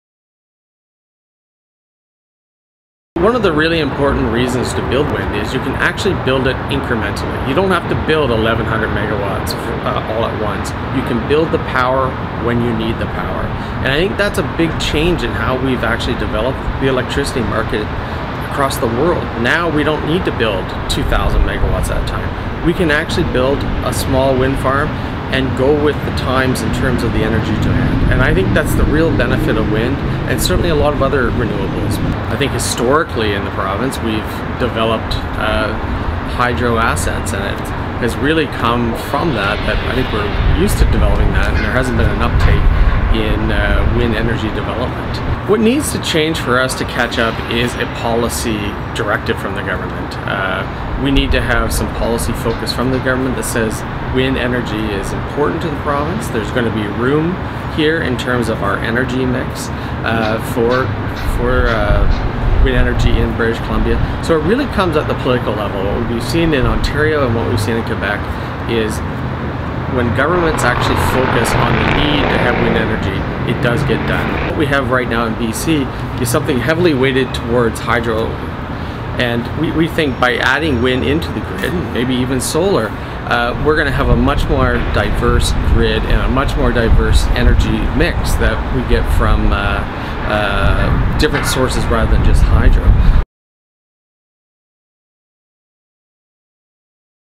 Is B.C. Hydro overlooking wind energy?
Experts say it would be a breeze for $10 billion worth of wind energy projects to inflate B.C.’s economy but worry the industry will remain in the doldrums with B.C. Hydro and the provincial government focused on the controversial Site C hydroelectric dam project.